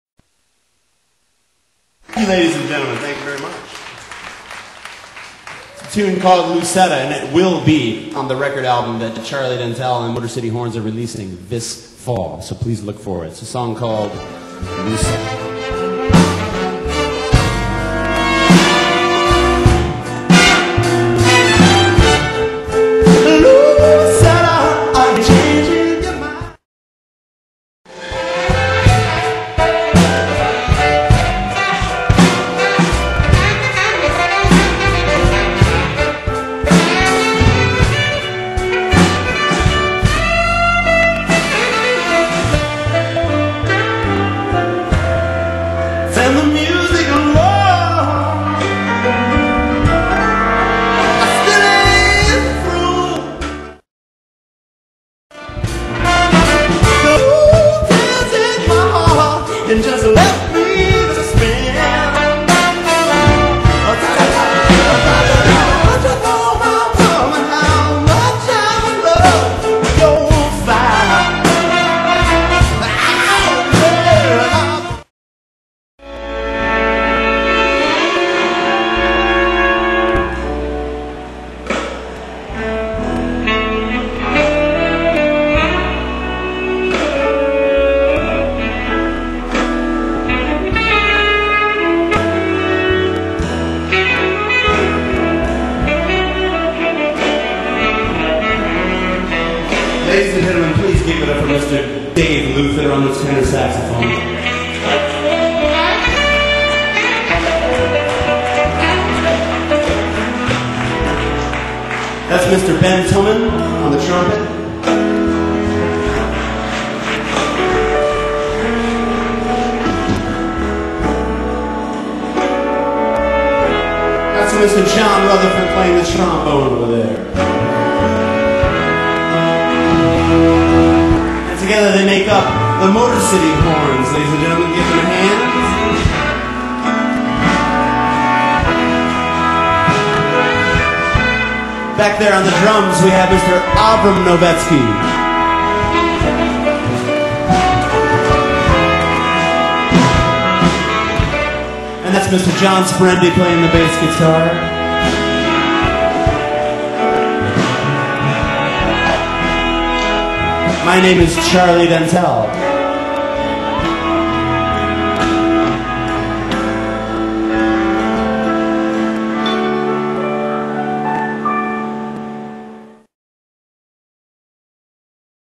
The Motor City Horns with Charlie Dentel at the Heritage Festival in Ypsilanti, MI (2001)
Charlie Dentel featuring The Motor City Horns at the Heritage Festival in Ypsilanti, MI 2001